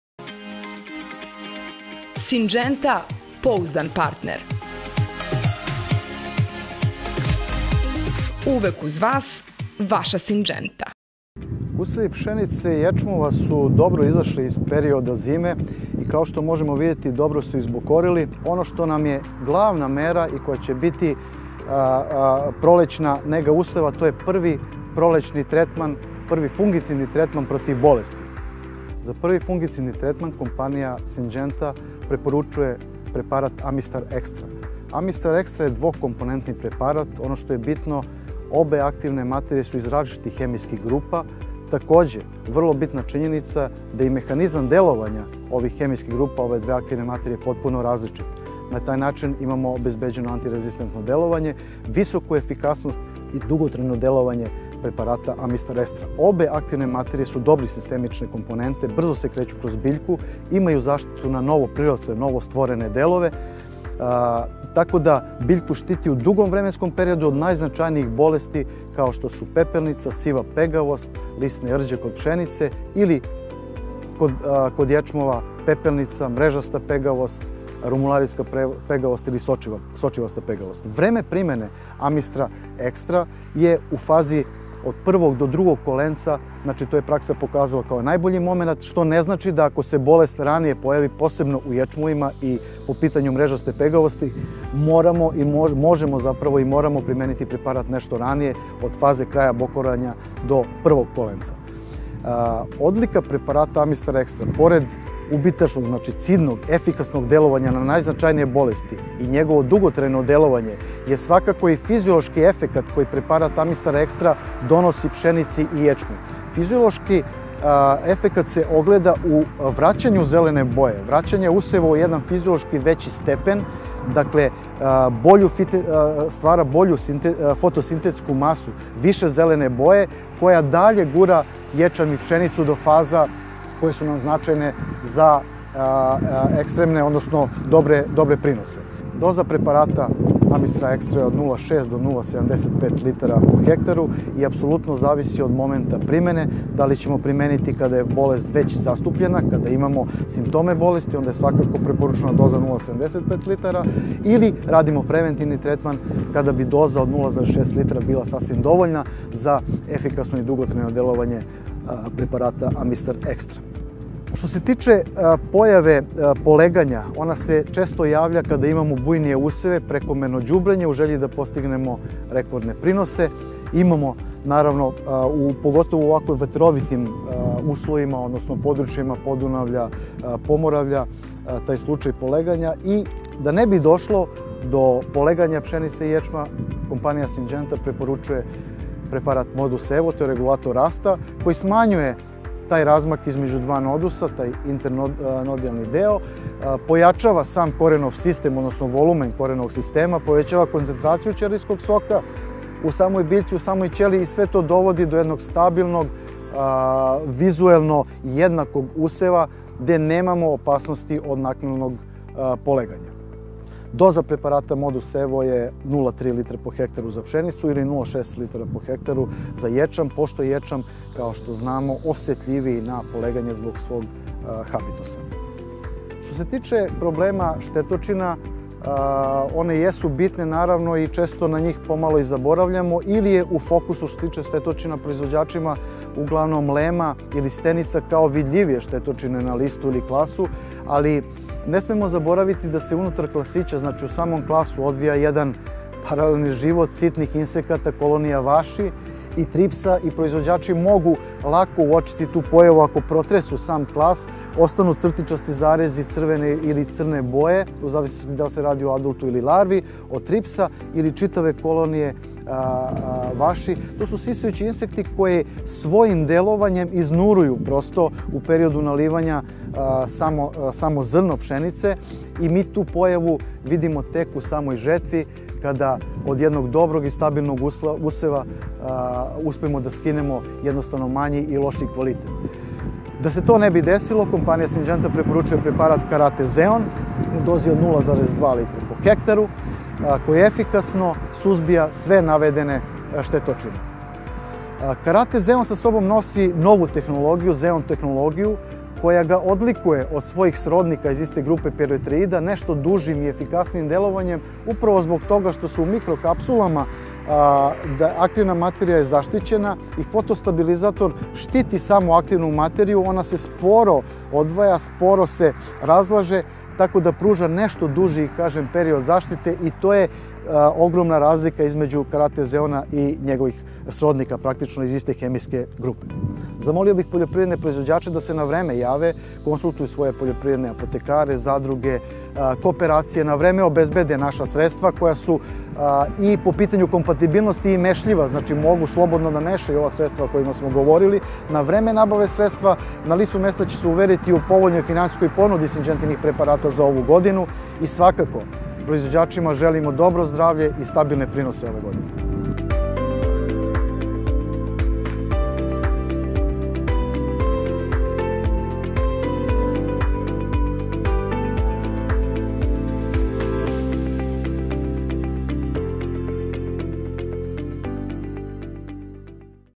Da bi održali lisnu površinu zdravom i tako sačuvali planirani prinos...
... potrebno je zaustaviti širenje bolesti s donjih listova na gornje. Prvim fungicidnim tretmanom znatno umanjujemo pritisak bolesti u fazi intezivnog rasta strnih žita, dok sa kasnijim tretmanima štitimo list zastavičar i klas koji su ujedno i nosioci prinosa.